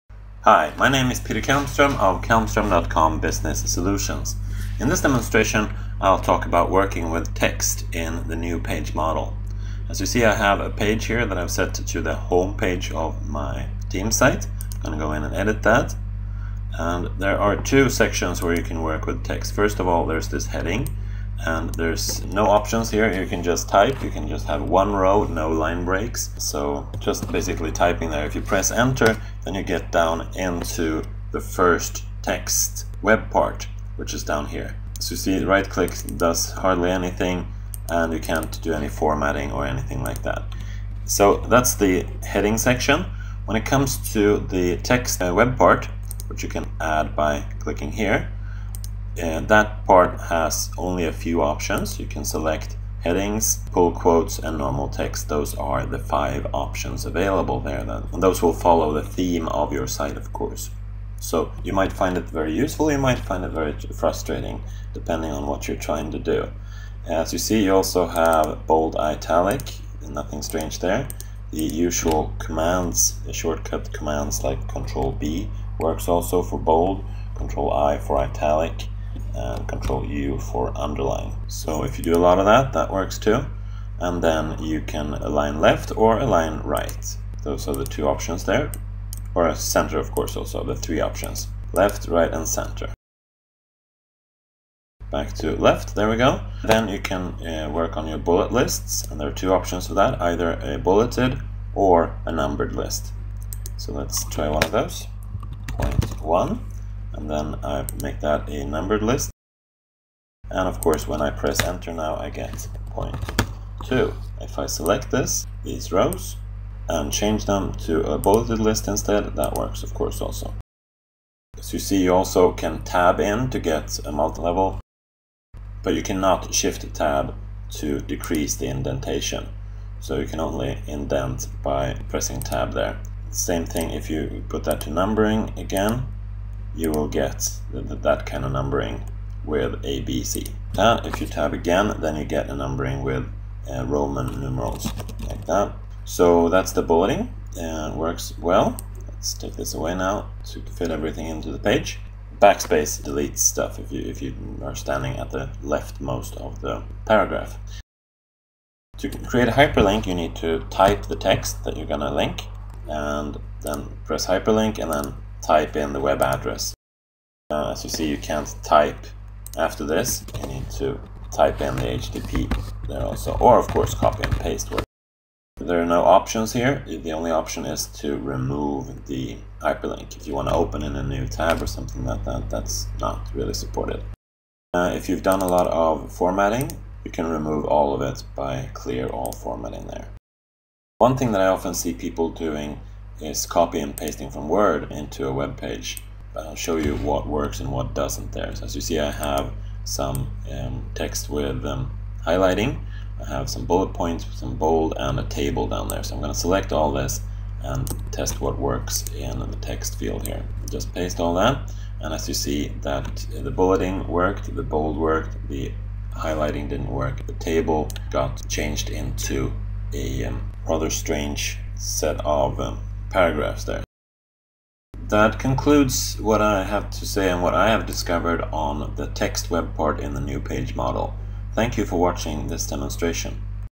SharePoint Online New Page Model - Text Web Part
Learn how to add text to a SharePoint Online new model page - limitations and possibilities